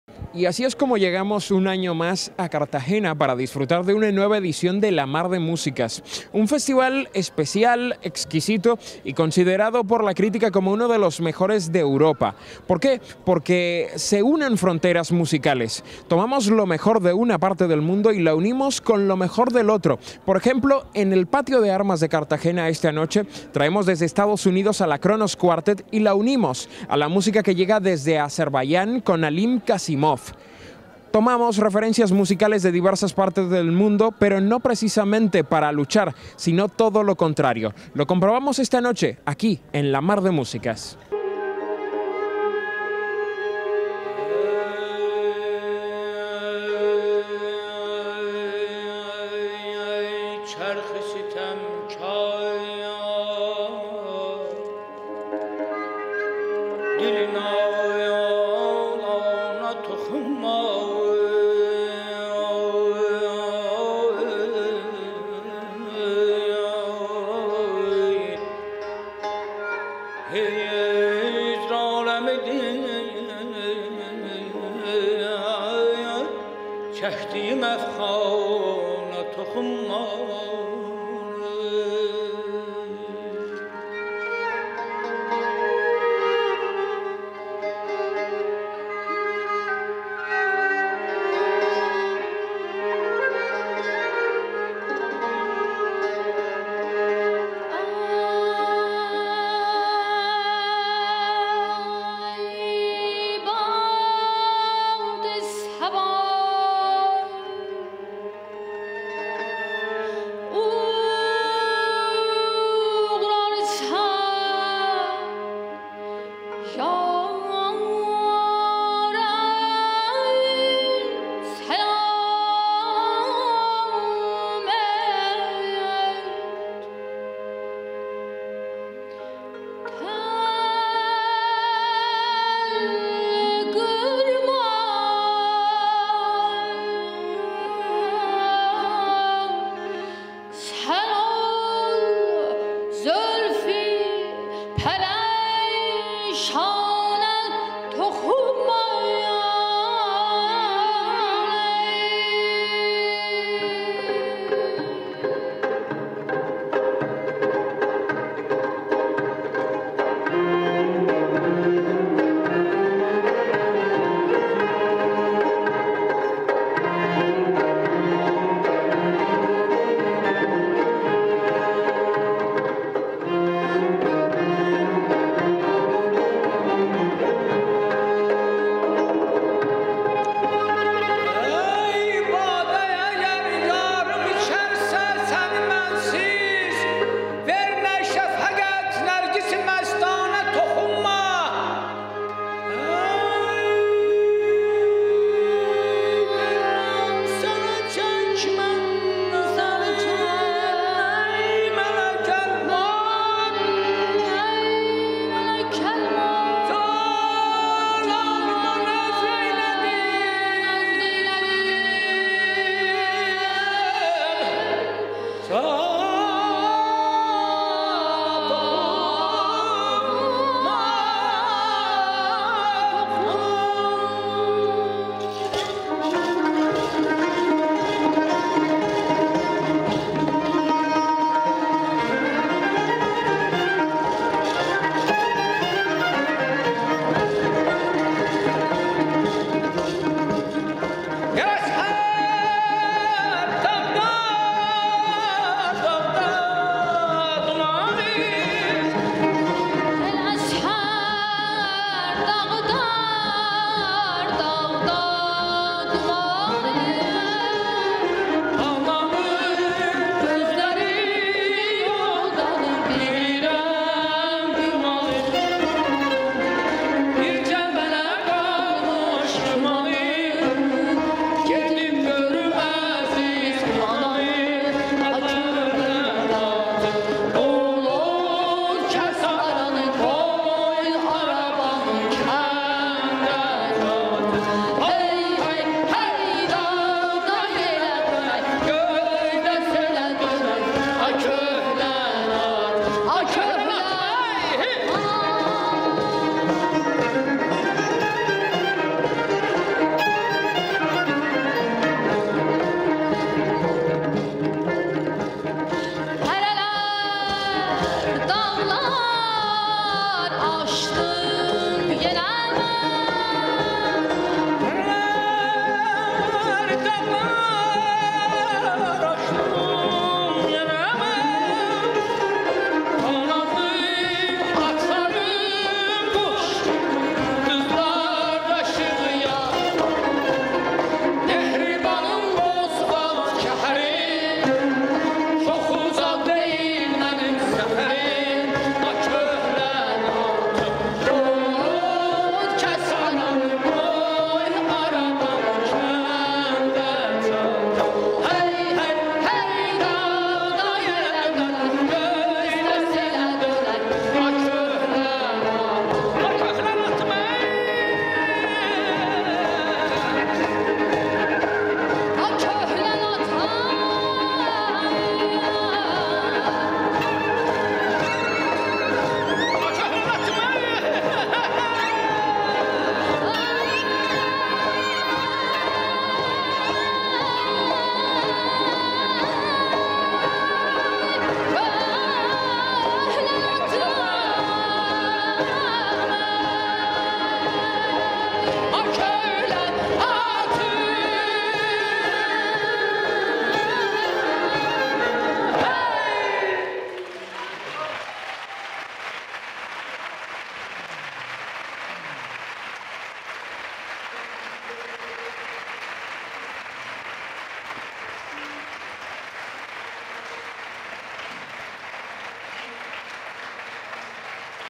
La Mar de Músicas 2010: El Kronos Quartet arropa a Alim Qasimov
El día 9 de mayo el Cuarteto del Tiempo visitó una vez más, y van ¡21!, el Théâtre de la Ville de París. David Harrington (violín), John Sherba (violín), Hank Dutt (viola) y Jeffrey Zeigler (chelo) se encontraron en el teatro de la plaza del Châtelet, a orillas del río Sena, con los músicos de Alim Qasimov. Un conjunto formado por el extraordinario cantante, su hija Fargana Qasimova (voz y daf) y tres músicos más de Azerbaiyán. Los arreglos de las obras interpretadas por el Kronos Quartet y el Ensemble Alim Qasimov son del azerí mientras que los arreglos de cuerdas destinados al Kronos los firma Jacob Garchik. El proyecto es un encargo del Aga Khan Trust for Culture -para la conservación y el desarrollo futuro del patrimonio musical de Asia central- y de la Fundación Columbia. Así sonó en el Patio de Armas de Cartagena.